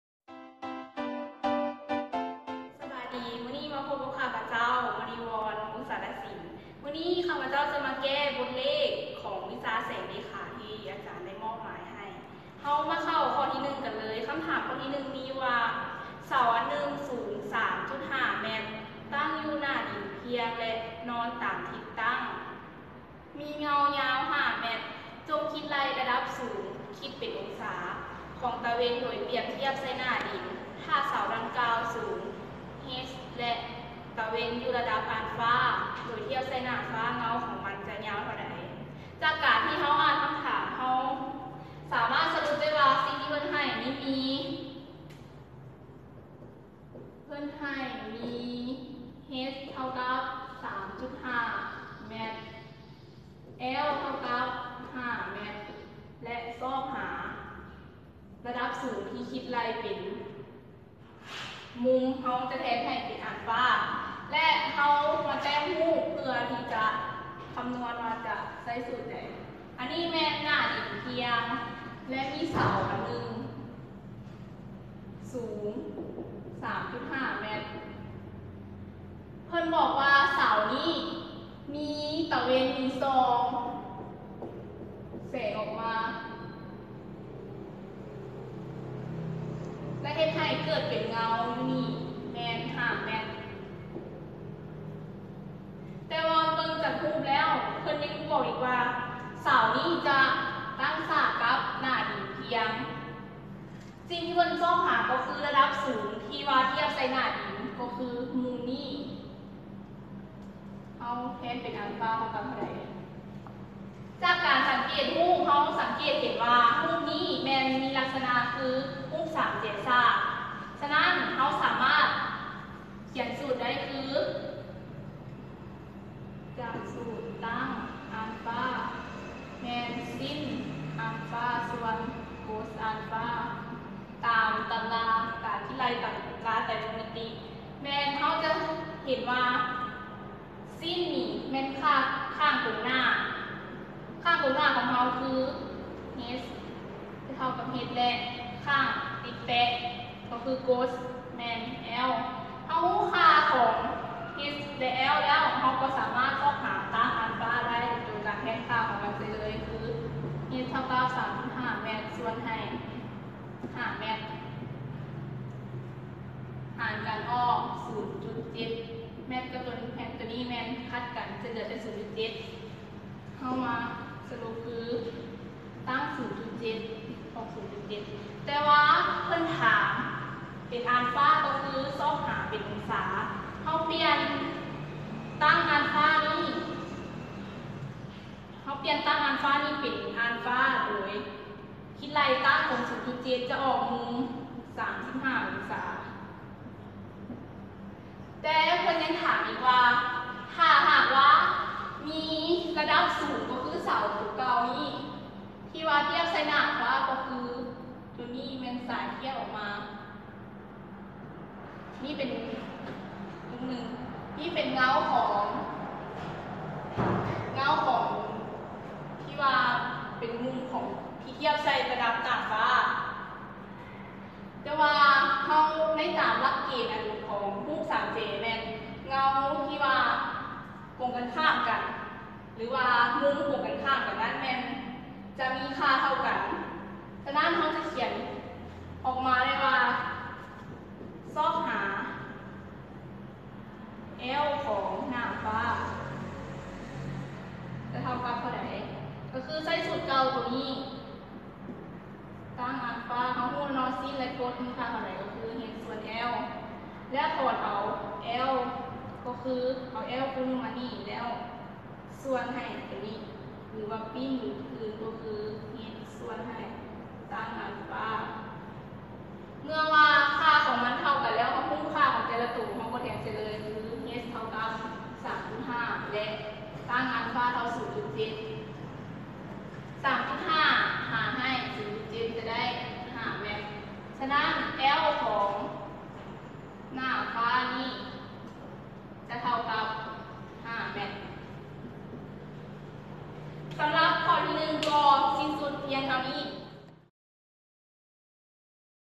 Ep1: ບົດແກ້ບົດເຝິກຫັດ ວິຊາແສງເລຂາ(ການເຄື່ອນຂະຫຍາຍຂອງແສງ) ຂໍ້1
ສະບາຍດີ...!! ຂ້າພະເຈົ້າ ນ.ມະນີວອນ ວົງສາລະສິນ ສາຂາຄູຟີຊິກປີ1